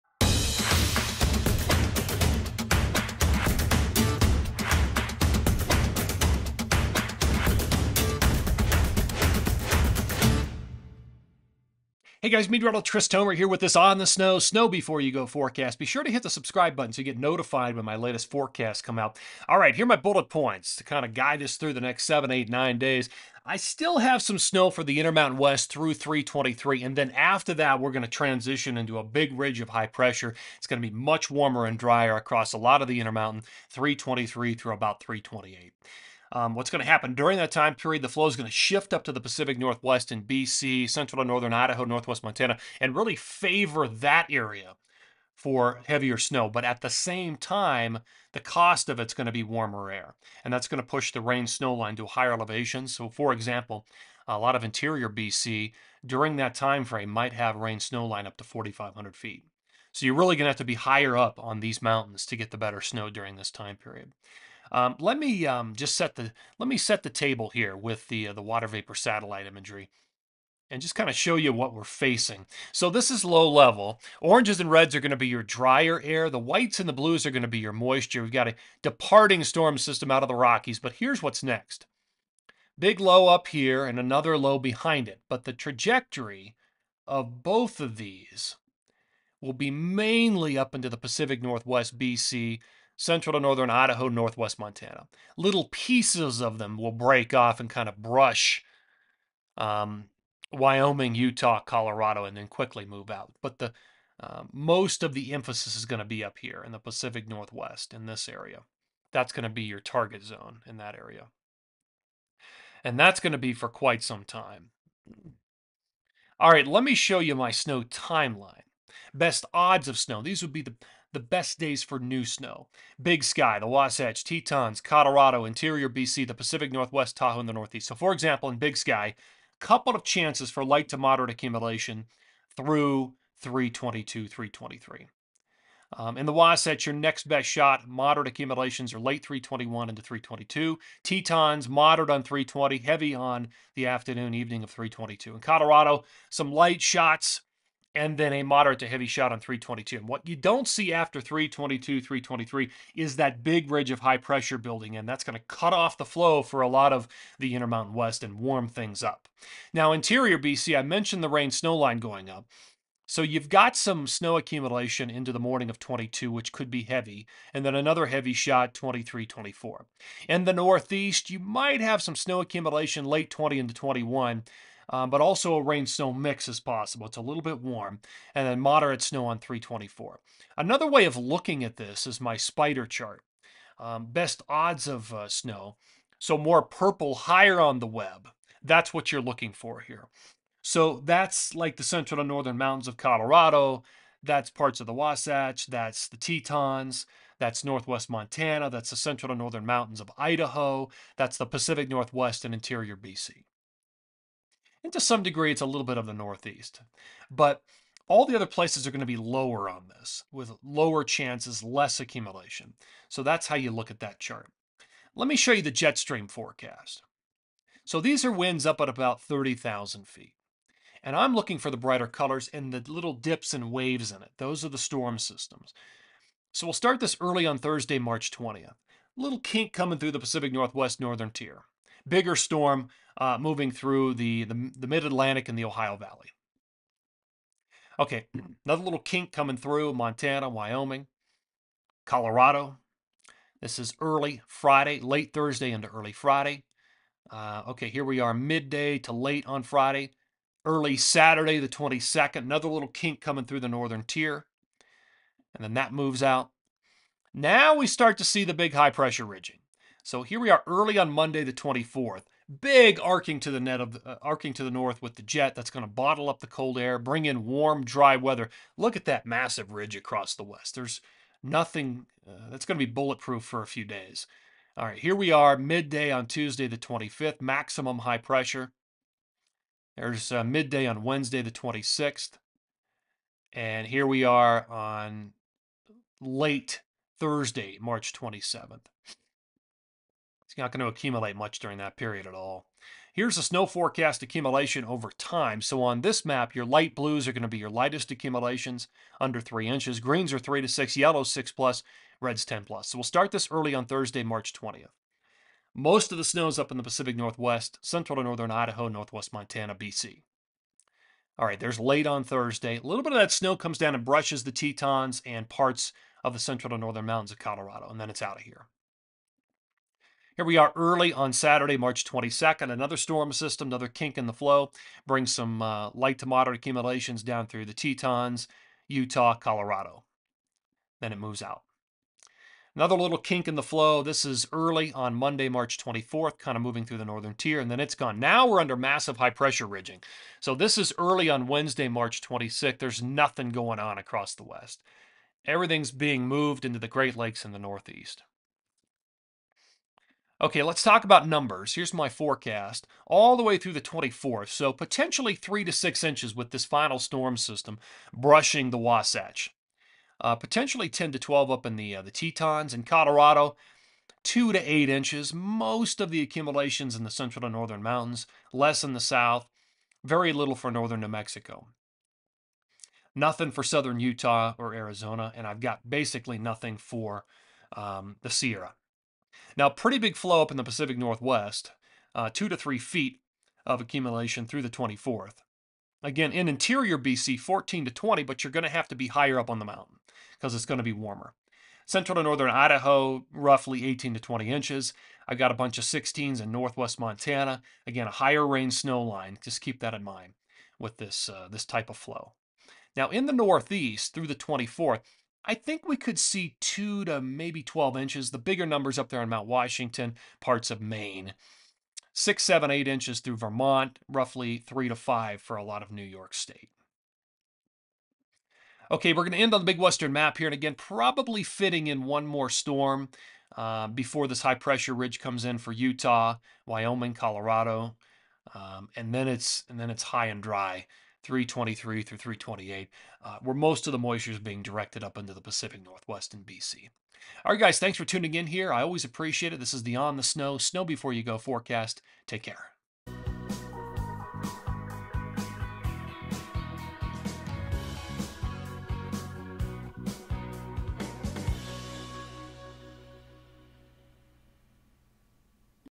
Snow Forecast for the Fourth Weekend of March
Meteorologist Chris Tomer shares the North America snow forecast for the fourth weekend of March and early next week. Read the North America snow forecast in its entirety, and past snow reports, on our